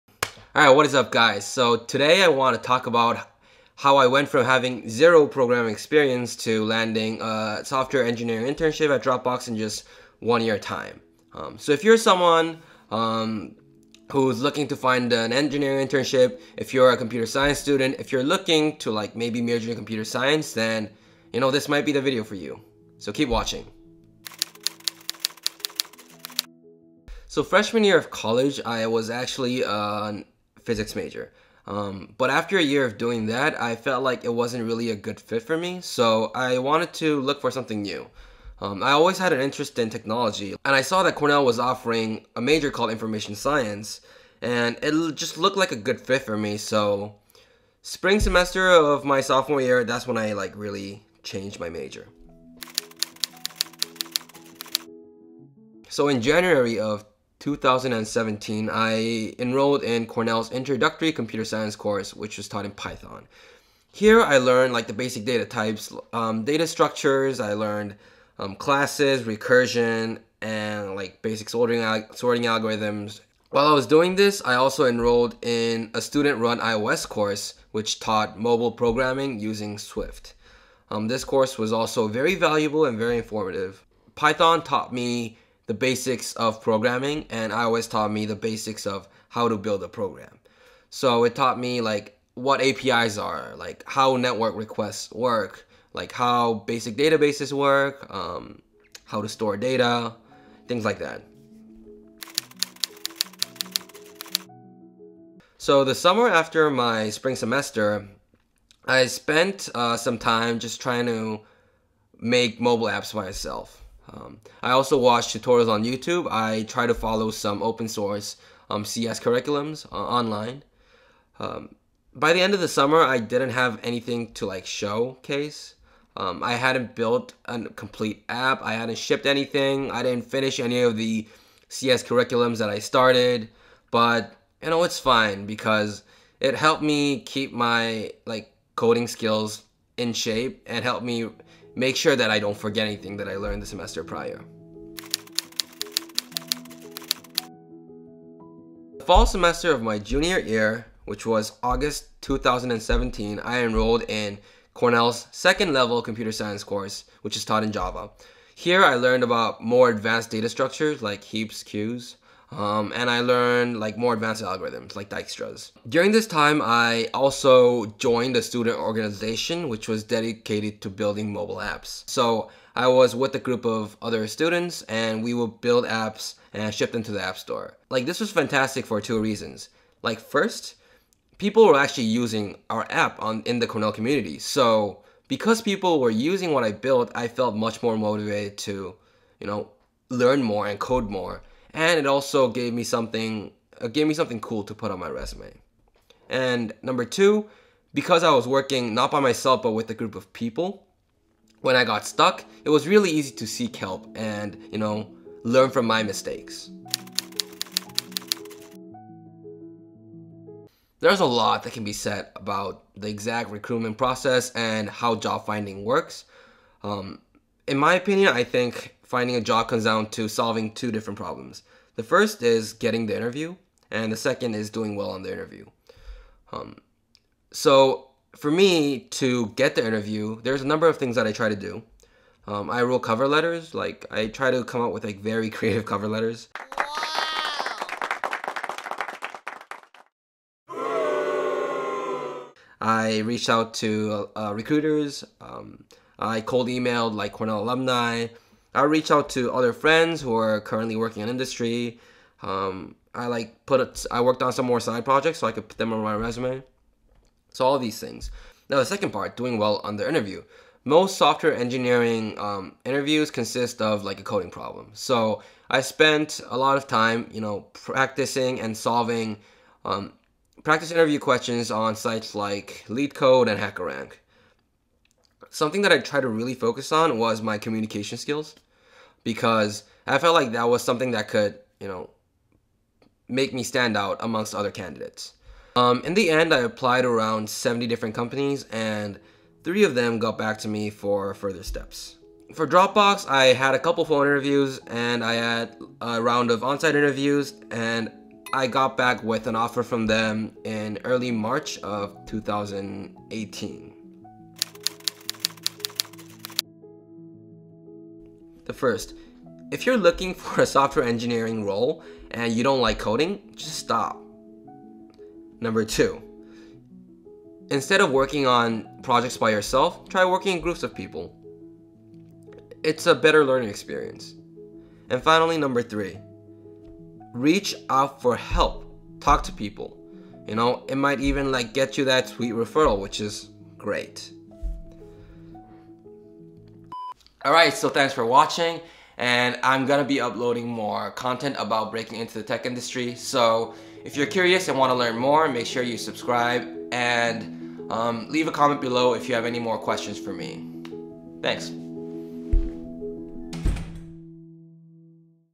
0 Coding Experience to Dropbox Engineering in 1 Year
In January 2017, I first learned to code. 14 months later, March 2018, I received an offer from Dropbox for their engineering internship program.

Here, I discuss what I did that made this possible :)